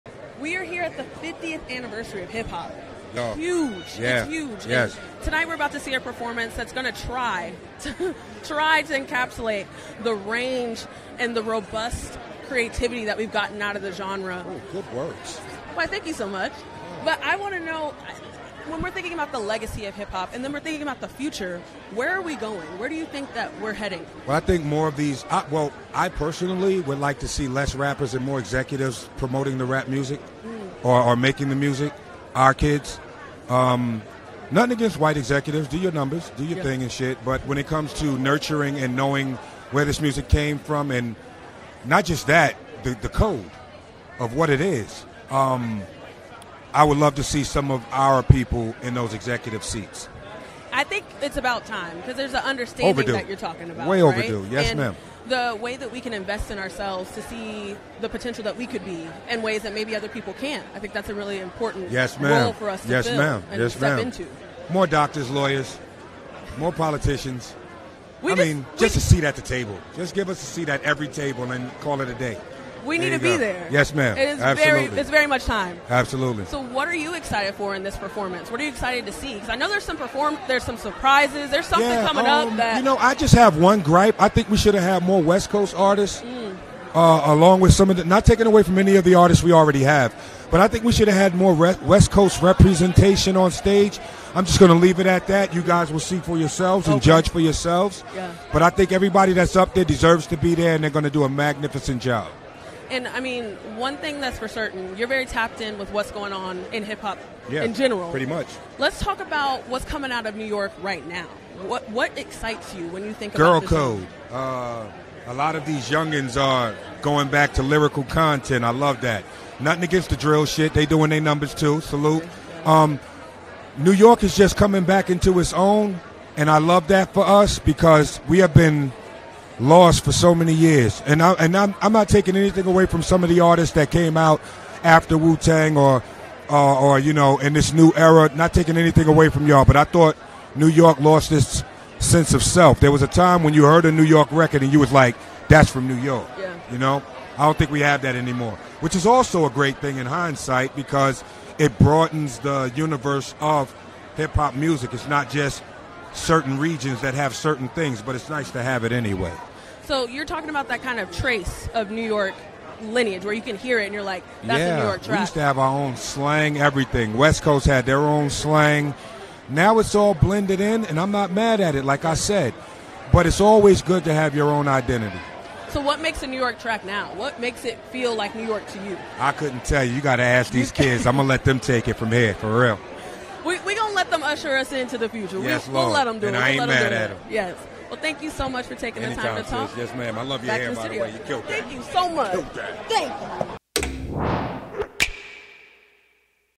2023 GRAMMY Awards Red Carpet: Method Man
Rolling Stone’s Delisa Shannon caught up with Method Man ahead of the show.